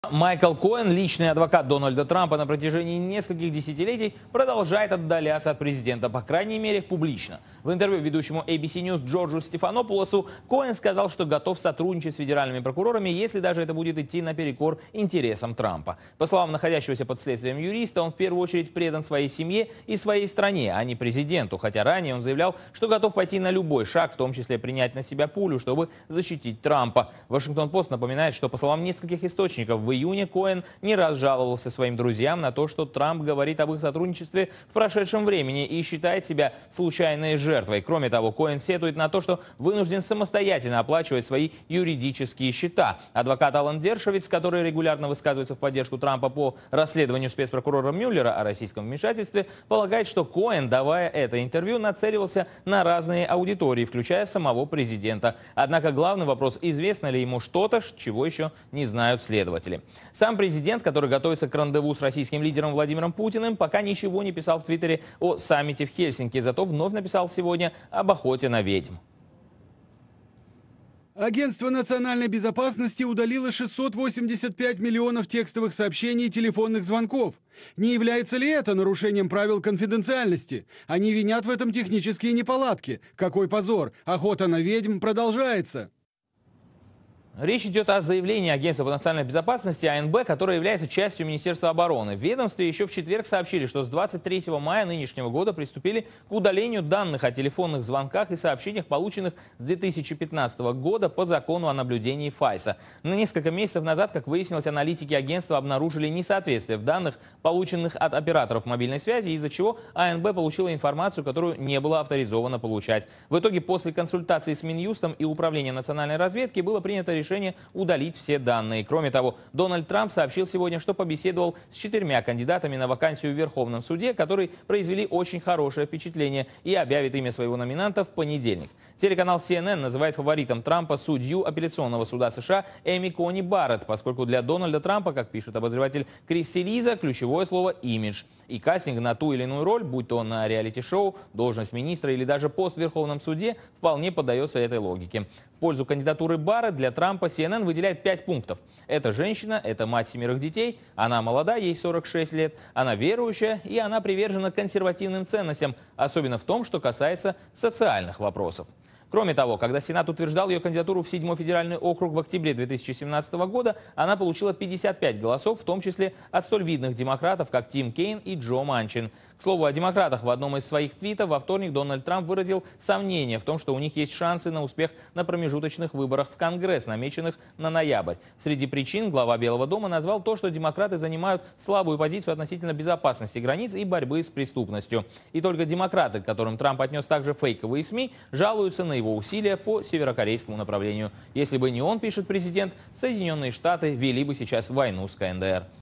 Коэн все дальше от Трампа
Личный адвокат Дональда Трампа на протяжении нескольких десятилетий Майкл Коэн выразил готовность сотрудничать с федеральными прокурорами, если даже это будет идти наперекор интересам президента США